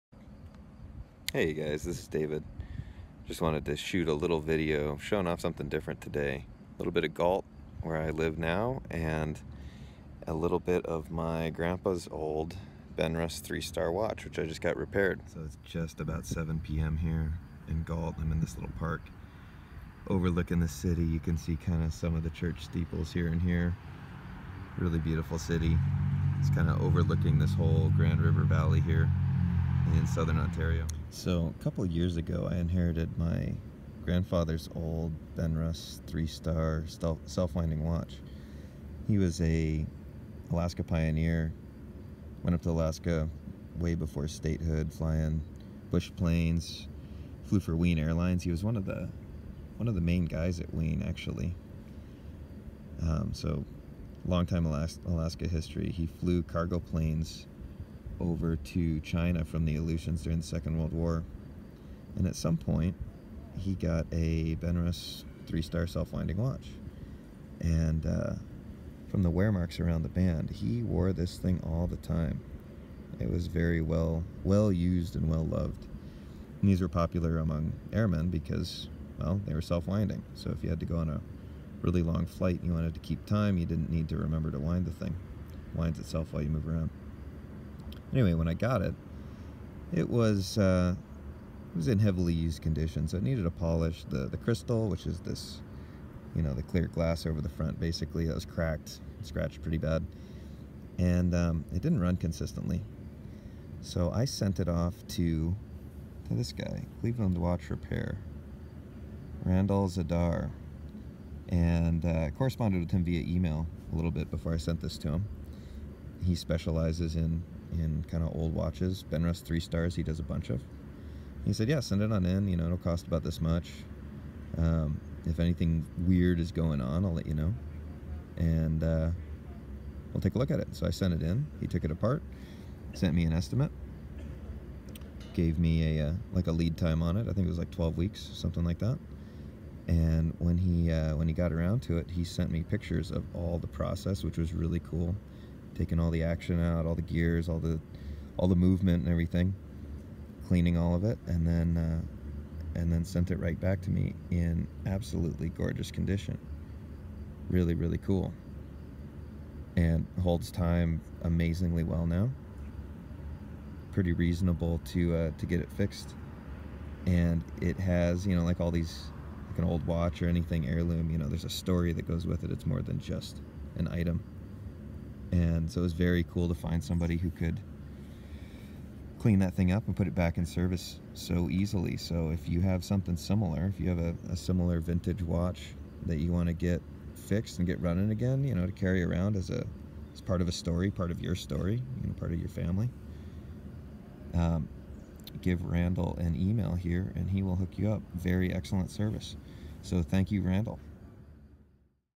Grandfather Jerry Bohms' Benrus 3 Star Watch - Restored to New
My Grandfather Jerry Bohms was an Alaska Pioneer, Aviator, and respected and loved member of the Fairbanks community. After he passed away, I inherited his Benrus 3 Star (or Tri-Star) watch. These watches are a self-winding Swiss mechanism that had final assembly in America. More importantly, it was part of his story as a pioneer and aviator.

I found Randall Zadar at Cleveland Watch Repair and contacted him to have the watched repaired to operating condition, cleaned, and generally restored. He did an absolutely fantastic job at a very reasonable price.

Since getting it back, I've been enjoying this connection to and reminder of where I came from. Thank you for your work on this piece, Randall.